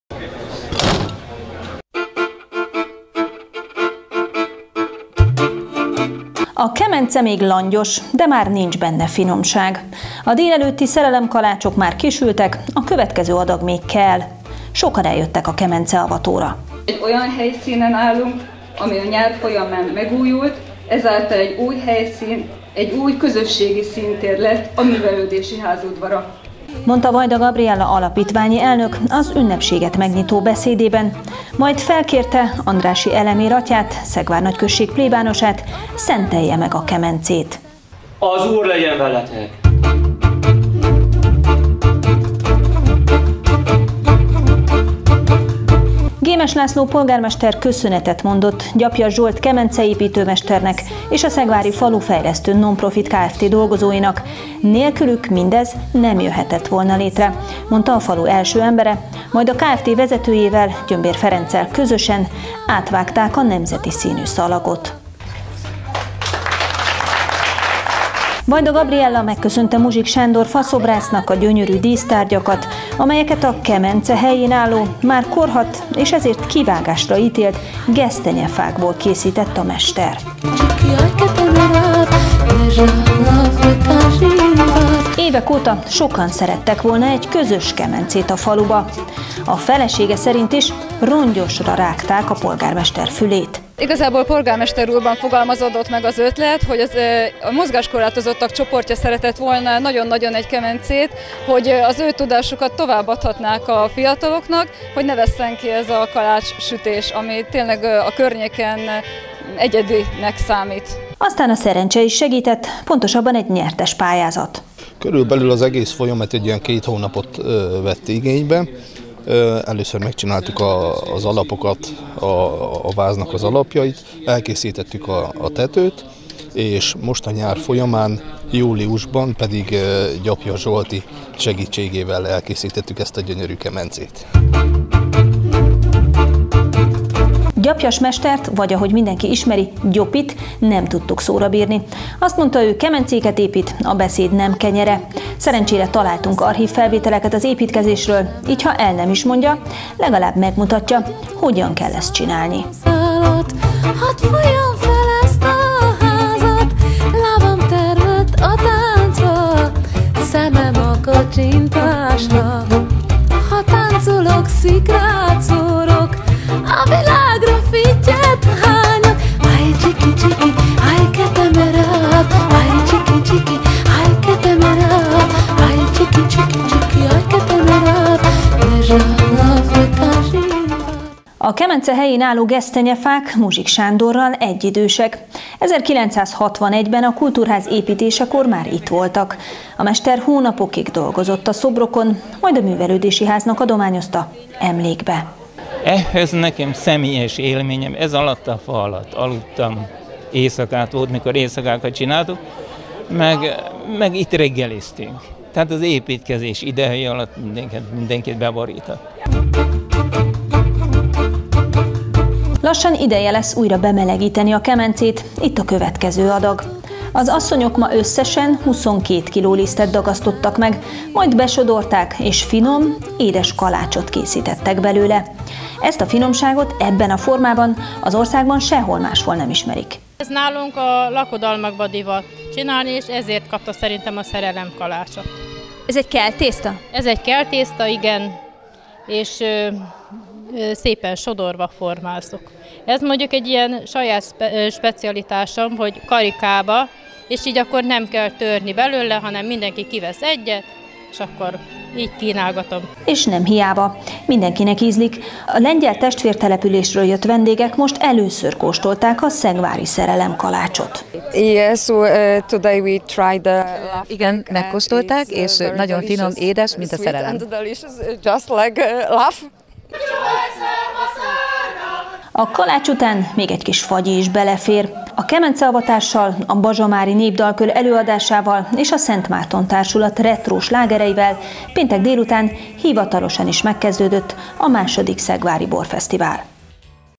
Szerelemkaláccsal avatták fel Szegvár első, közös kemencéjét
A polgármester kitalálta, a mester megépítette, a hozzáértők befűtötték, az asszonyok megsütötték, a vendégek pedig mind megették. Találják ki mire gondoltam! Vagy nézzék meg a következő riportot.